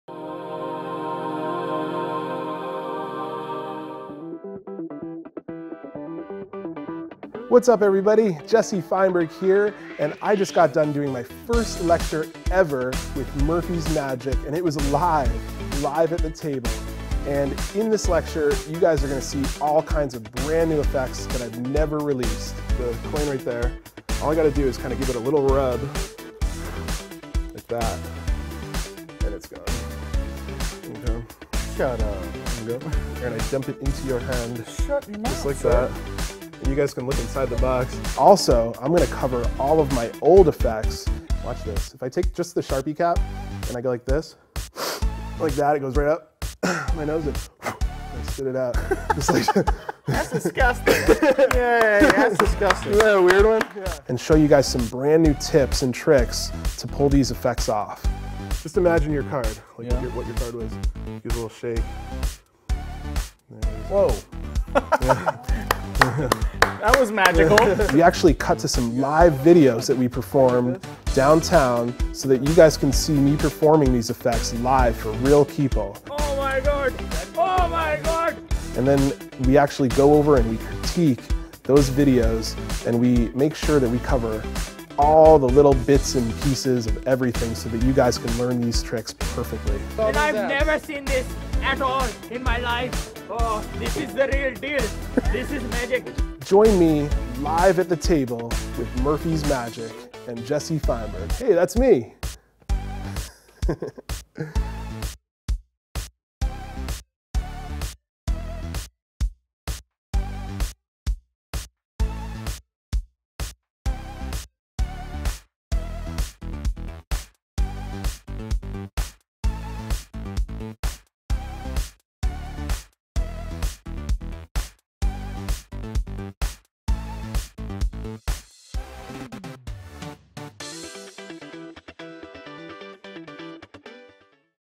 At the Table Live Lecture Jesse Feinberg
World Magic Shop DO NOT SELL Every product listed anymore. for the items we sell. the Table Live Lecture Jesse Feinberg

Jesse Feinberg is a true creative force in the world of magic. Having consulted for some of the top names in magic including the one and only Criss Angel, as well as produced mind boggling magic with the likes of Paul Harris, Jesse definitely has a story to tell. His magic is unique and baffling, with methods that will make you shake your head in disbelief. So prepare yourself as Jesse brings you into HIS world of magic. With effects such as:

Rip Off - A BRAND NEW, impromptu SIGNED torn and restored card.

X-Finger - Real permanent ink VISUALLY travels to a new location.

BLUR - A BORROWED iPhone screen becomes impossibly blurry.

McStraw - A super visual effect that uses an ordinary fast food straw.

Spell-O-Change - Make random scribbles on the cellophane of a deck of cards VISUALLY form into a written prediction. This one is EXTREMELY visual.

Coin Thru Box - Cause a BORROWED and SIGNED coin melt right through a regular card box.

Bits and Pieces - This is a section Jesse calls his "Inspiration round". This will be a discussion, demonstration of The Floating Card, The POP Card Change, The Ambitious Card and a NEW Mercury Card Fold.

PLUS! - Jesse will perform and explain some of his hit effects including Wedge, YaYa and Animate & Restore.